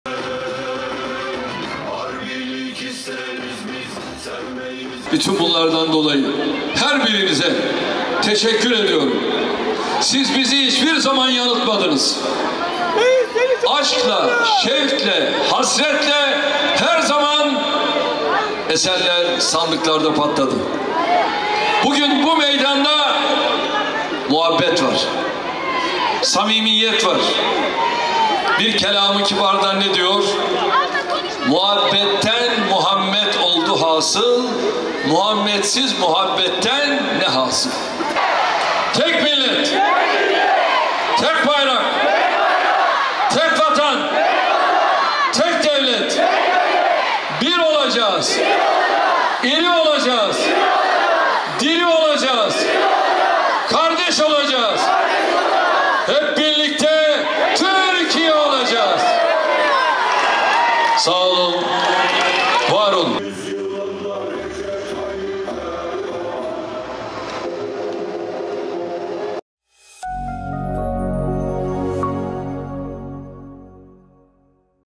土耳其總統拚連任 造勢現場如演唱會 | 中央社影音新聞
土耳其總統艾爾段昨天前往伊斯坦堡為競選連任造勢，支持者提早1小時沿路等待總統車隊，賣命爬上台階就是為了一睹本尊。總統登場時更是集體瘋狂，合唱歌頌艾爾段的歌曲，現場可比巨星復出演唱會。